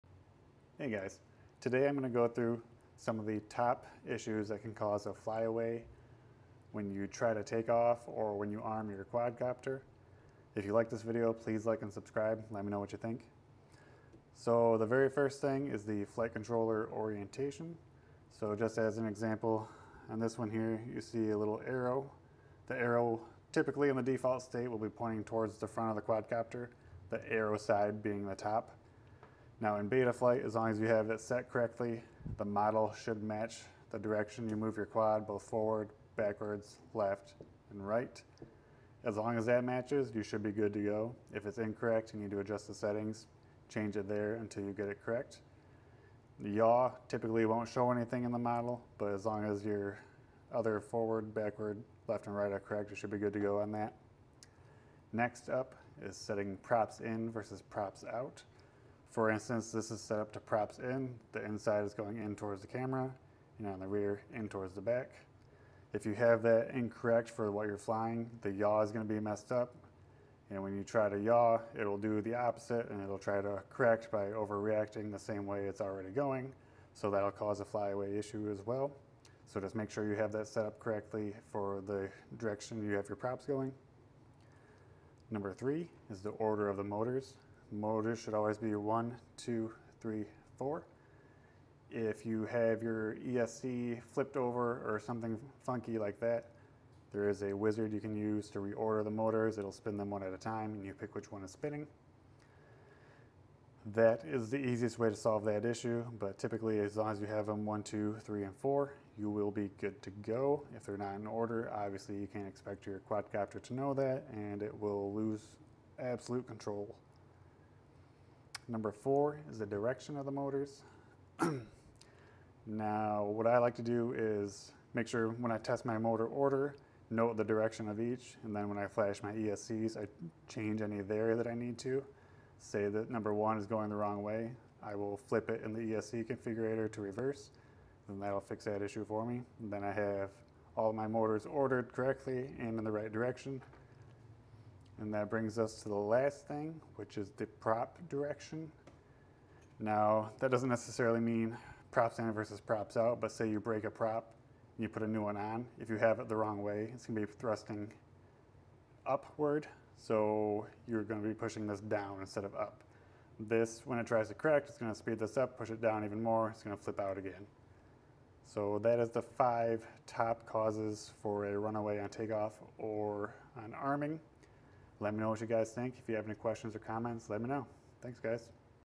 Drone Flipping Out: 5 Issues You Need to Know
0:00 Intro
0:14 Flight Controller Orientation
0:55 Props In / Props Out
1:25 Motor Order
1:58 Motor Direction
2:25 Propeller Direction

Does your quadcopter flip out when you arm it or try to take off and say "fly away"? Check this out and find a solution!

- Hobbymate D6 Duo
- Vifly Series Board - Perfect or 1s tiny whoops
- Gopro HERO11
- Walksnail Goggles X

Check me out on Linktree! More to come!